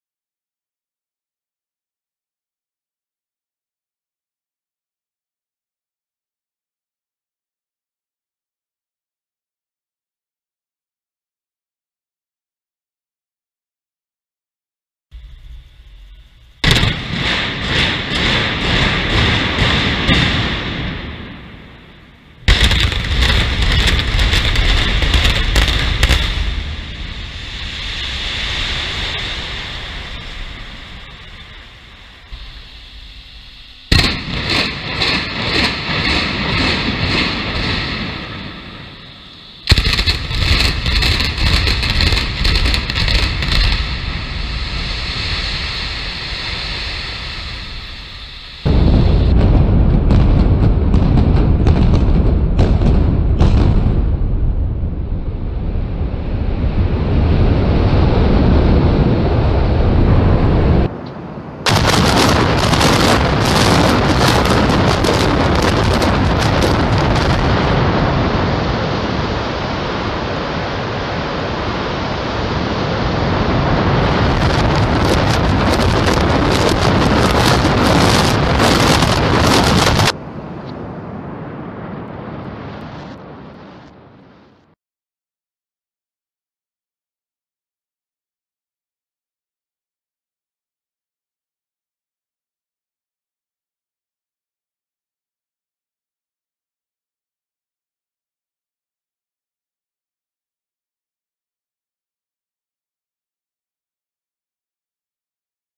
Jacksonville Old City Hall Annex--Controlled Demolition Inc.
Watch as the old City Hall Annex is brought down by Controlled Demolition Inc.